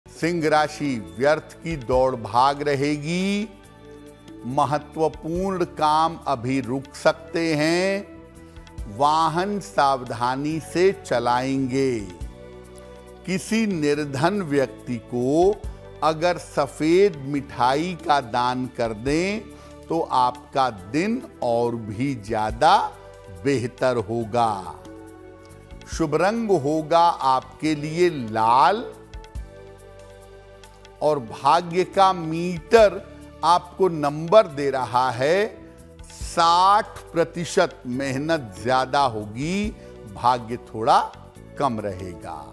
Aaj Ka Singh Rashifal 26 July 2024: वाहन सावधानी से चलायें | Leo Horoscope Today
Aaj Ka Rashifal Singh Rashi 26 जुलाई 2024: सिंह राशि के जातकों का ज्योतिष के अनुसार, वाहन सावधानी से चलायें. व्यर्थ की दौड़ भाग रहेगी. महत्वपूर्ण काम अभी रुक सकते हैं. सफेद मिठाई का दान करें. आज का शुभ रंग लाल है.